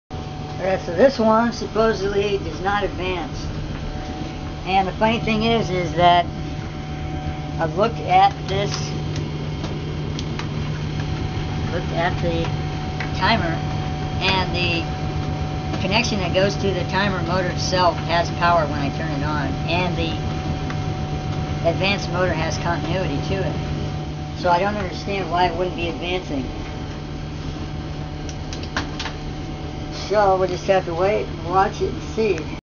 timer not advancing
i check for power to and continuity of the advance motor, in this case seems good but still not advancing, Timer should be replaced,